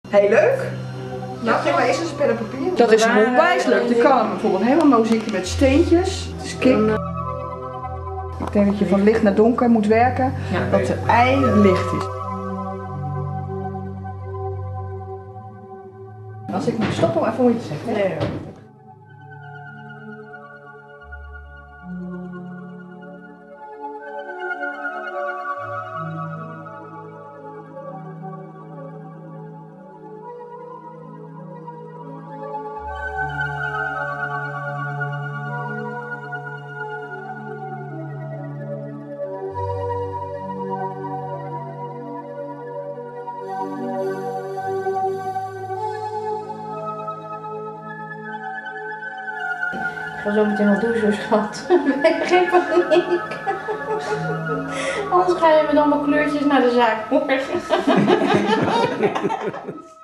BELLYPAINT in one minute.
Bellypaint by Yvon Twaalfhoven in one minuut.
Look how she creates a painting on a beautifull belly 2 days before giving birth.